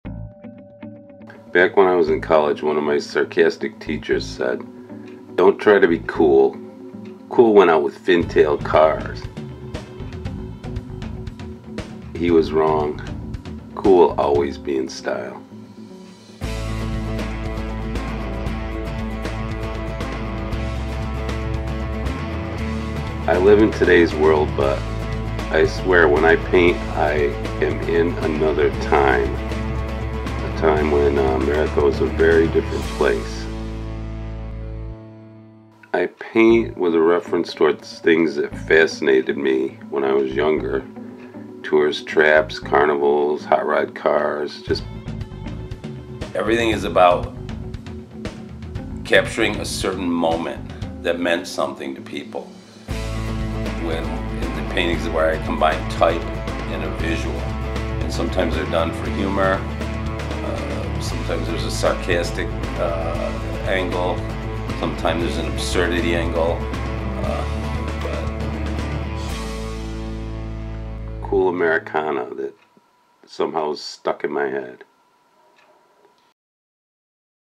Retro Art by artist Elliott Mattice
A look at the vintage/pop retro art and thoughts of American artist Elliott Mattice. Mixing hand rendered type and obscure visuals, his unique visual approach captures a lost era. This video contains paintings, graphics, illustration and interview clips from the artist. Clients include actress Mamie VanDoren, rocker Marky Ramone, Dinosaur BBQ and many others. His art has been included in "Art of Modern Rock" book, the Museum of Design Atlanta exhibition of poster art and is avidly sought out by private collectors.